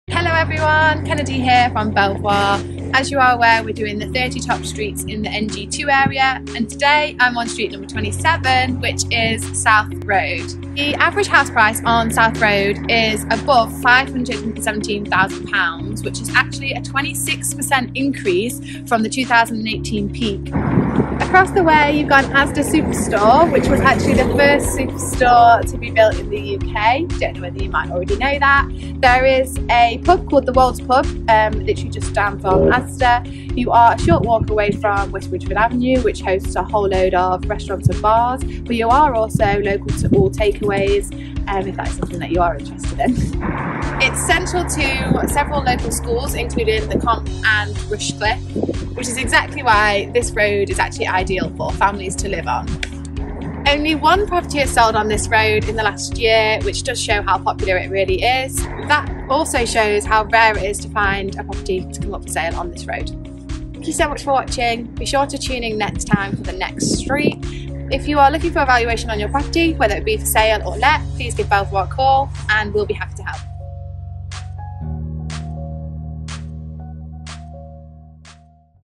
Belvoir! West Bridgford: Top 30 Streets - Street Number 27
We are here with number 27 in our top 30 streets in the NG2 area!

Do you have a guess as to what streets will be in our list? Have a guess in the comments below